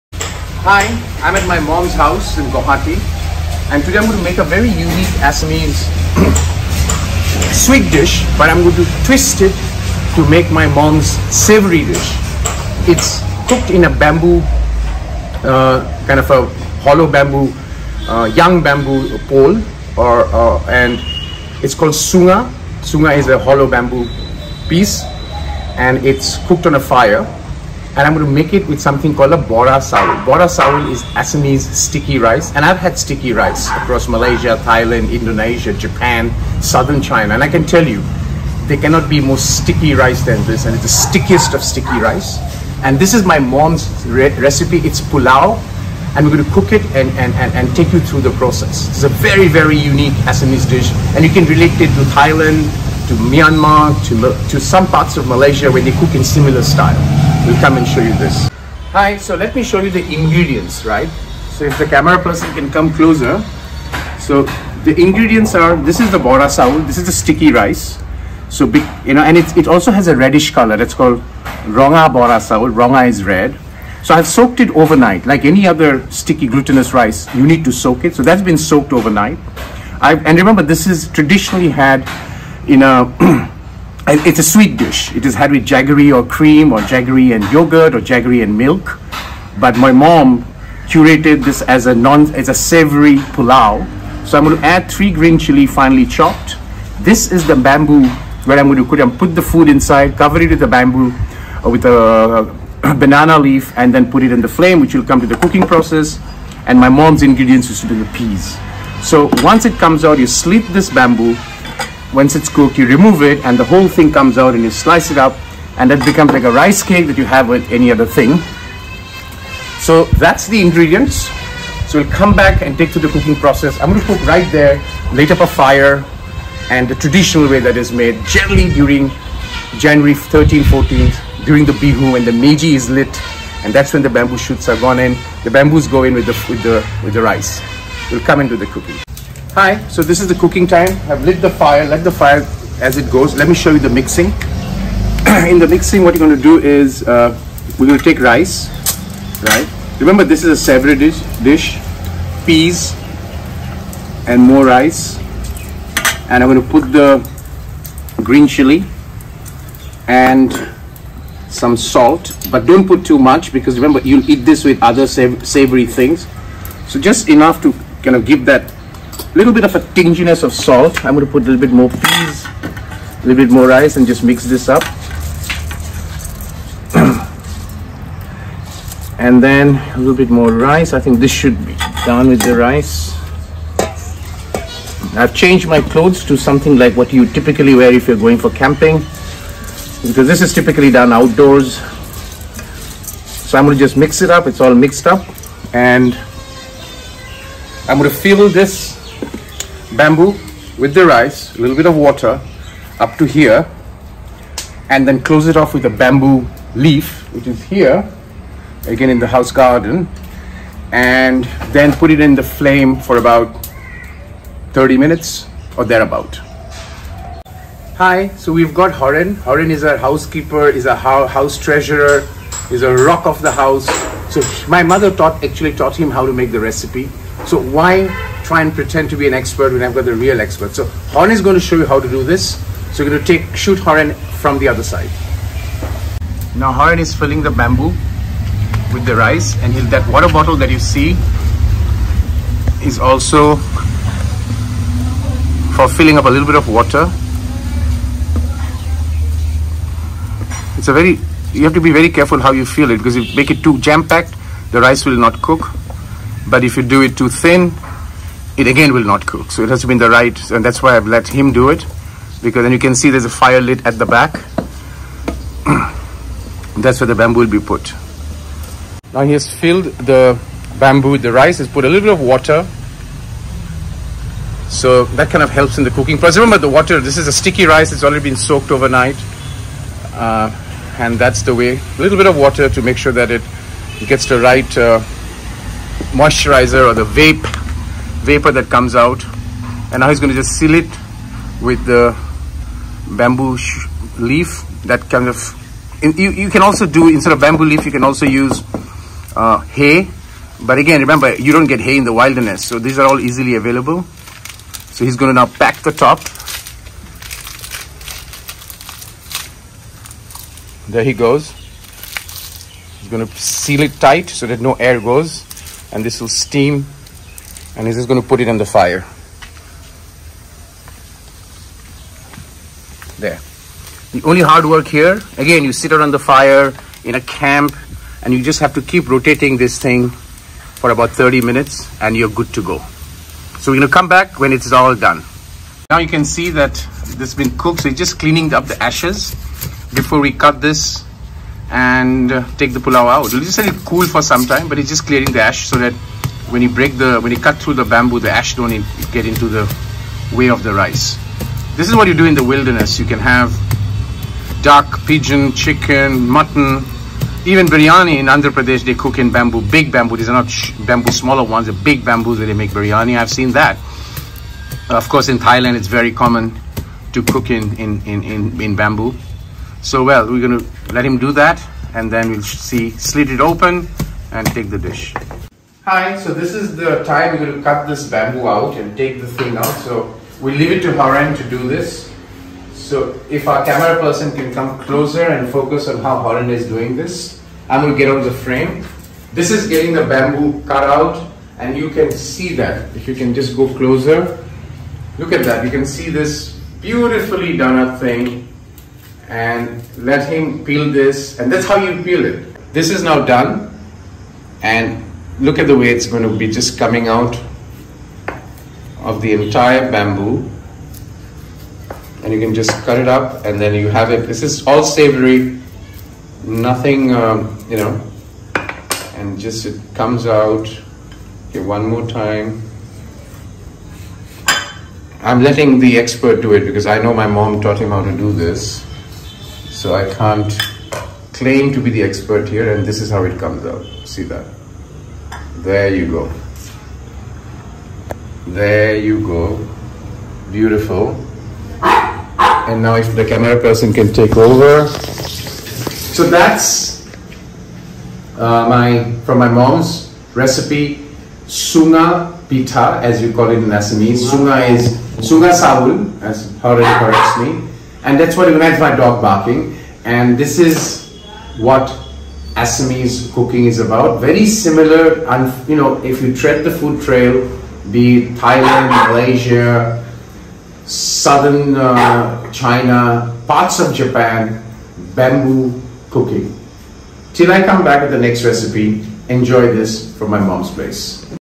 Bordo's Kitchen - Bamboo Pole Pulao [Bahar chungar pulao]
A traditional Assamese sweet dish with a twist from my Mom's curation into a savory dish
Ingredients
1) Sticky rice (100gm) soaked over night
2) Young 1.5 foot Bamboo pole with one side open
3) Peas (100 gms)
4) 3 green chilis finely chopped

Please see the video for cooking instruction